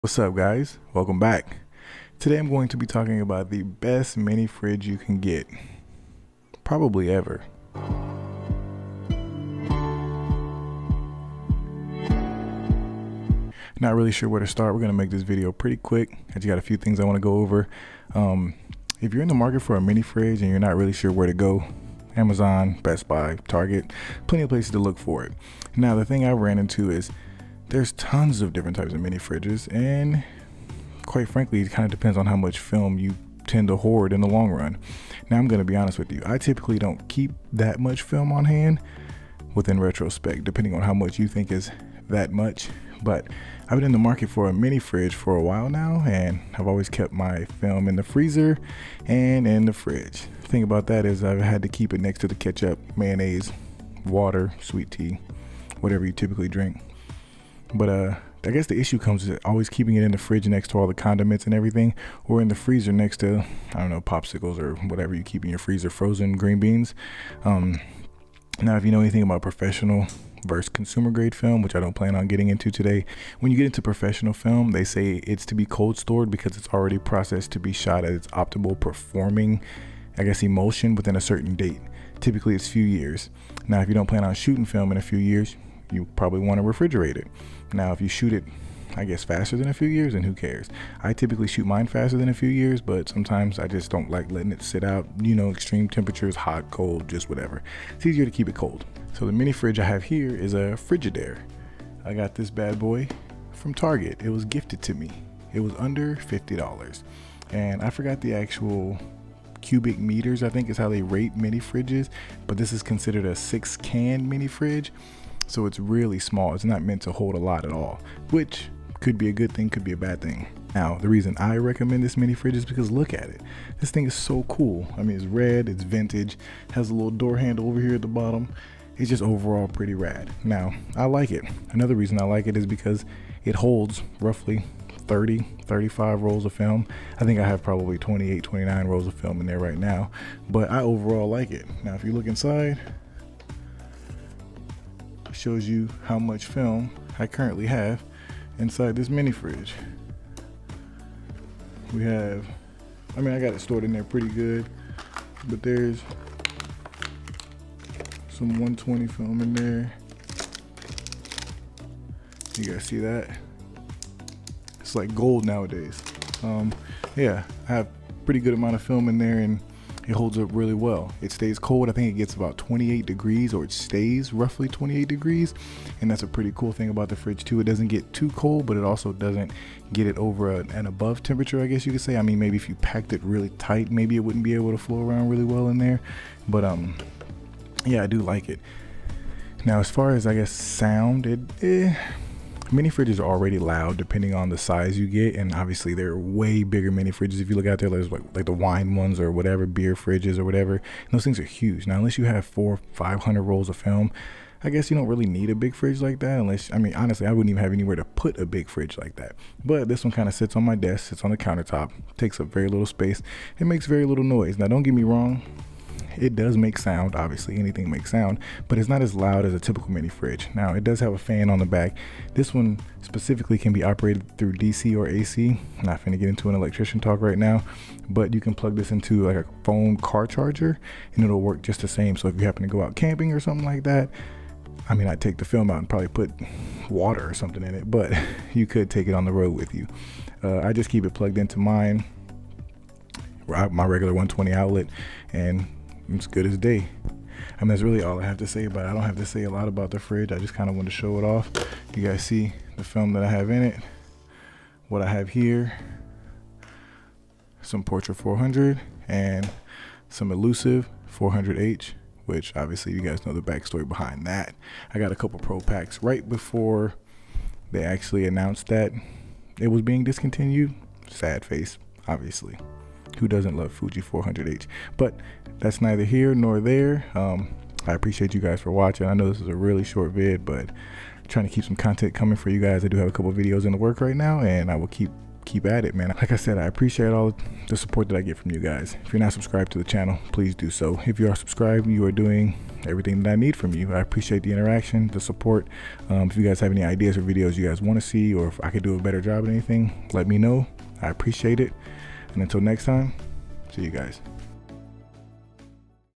The Best Mini Fridge for Film
A quick video of the new mini fridge I got specifically for film. Nothing informative going on here today

link to the fridge